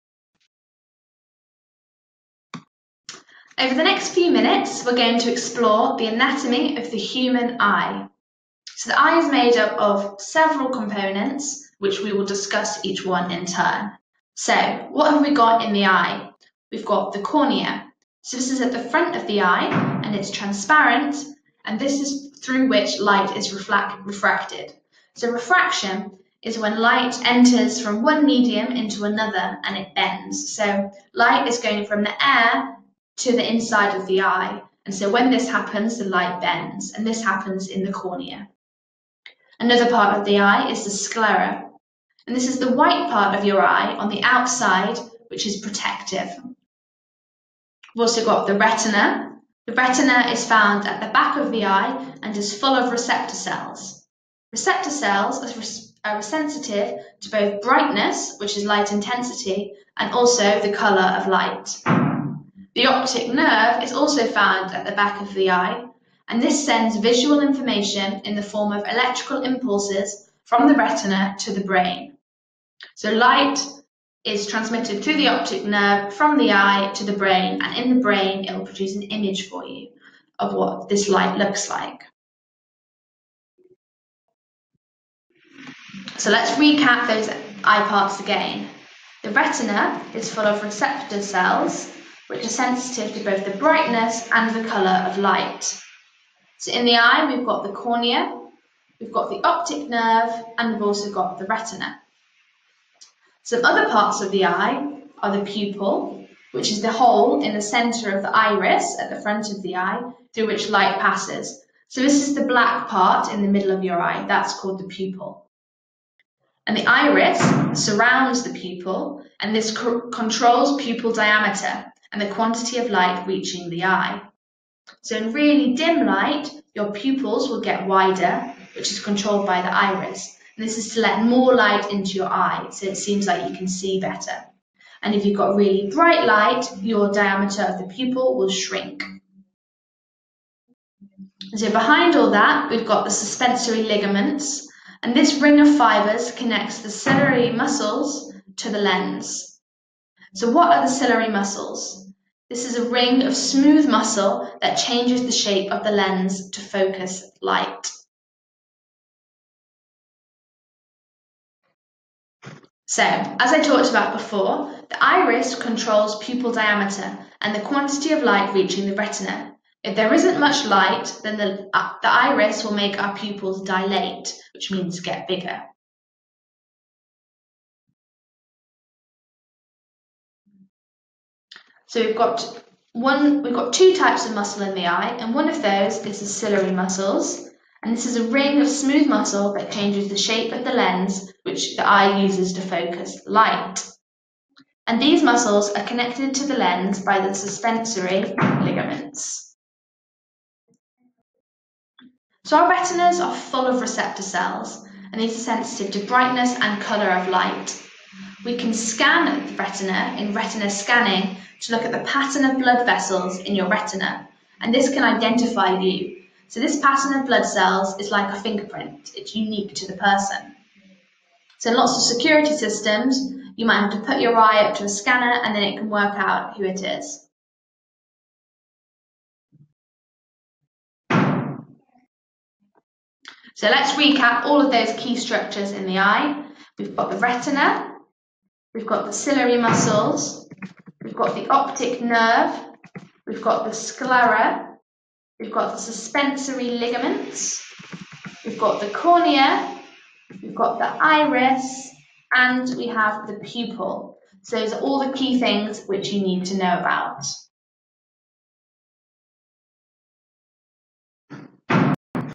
Eye Anatomy - Seneca - Learn 2x Faster
Liz looks at Eye Anatomy for your GCSE Biology exam.